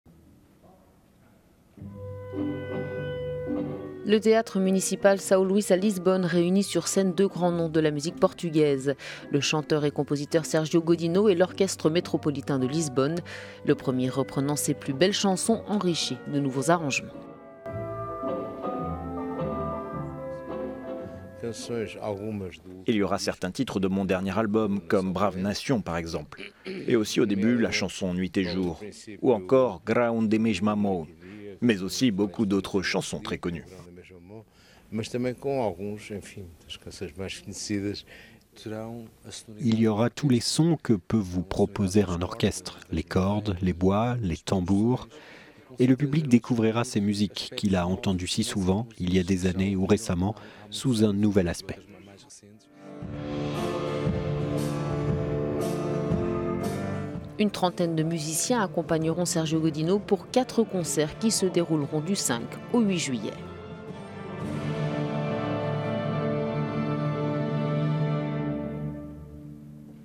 Sérgio Godinho et l’Orchestre Métropolitain de Lisbonne ensemble sur scène
Quatre concerts sont programmés du 5 au 8 juillet au Théâtre Municipal São Luiz à Lisbonne. …